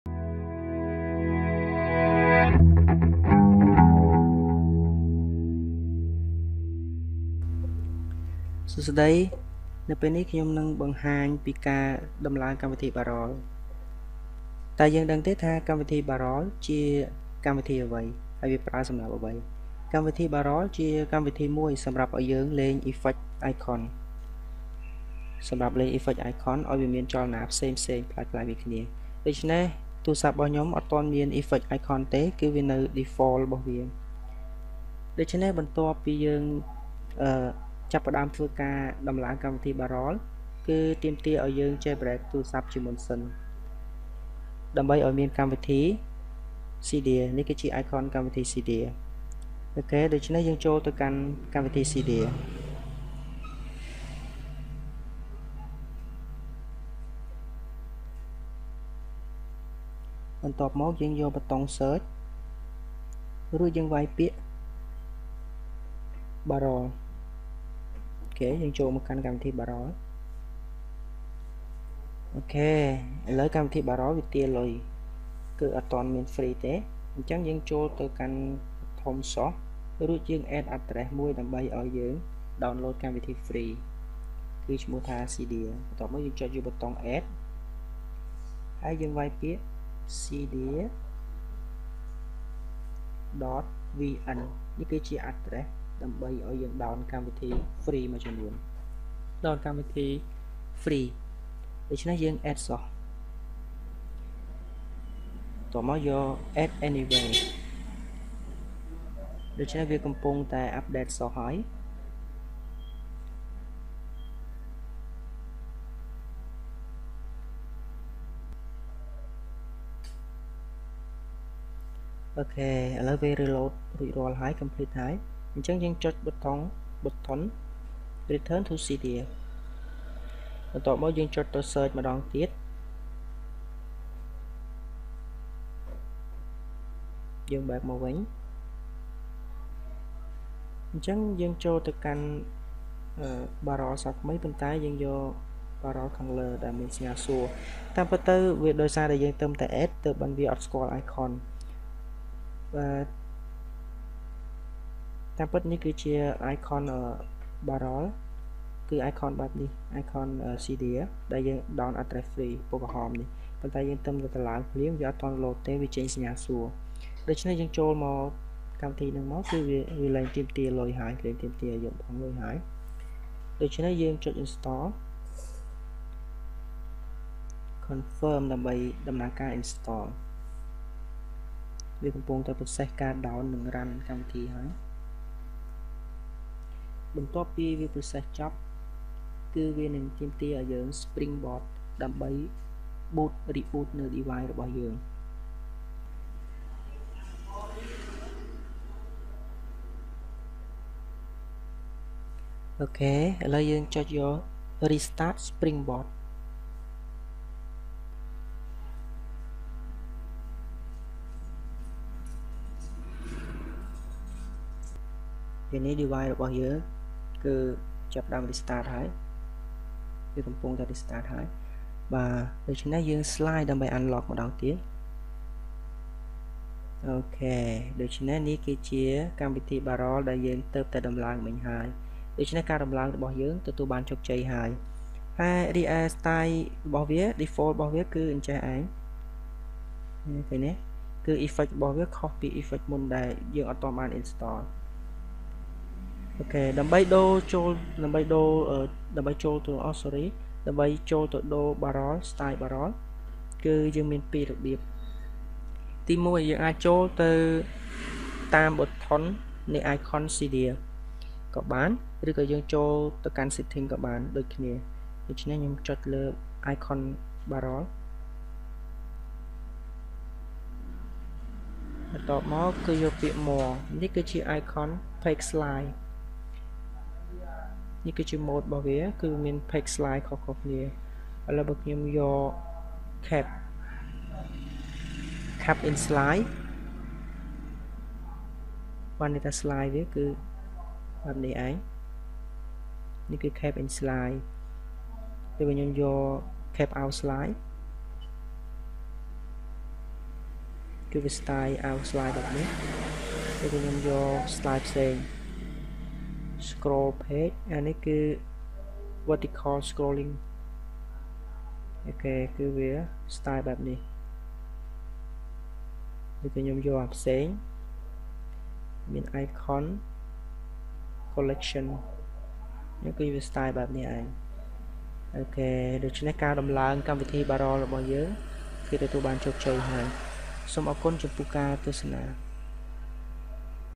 [iOS 7.1.2] How to install Barrel on iphone, iPad mini, iPod || របៀបលេងស្ដាយ iCon សម្រាប់ iPhone
Hi,
Today I will show you how to install Barrel for ios 7.1.2 on iPhone.
Do you know or can you use it?
OK, let's get started!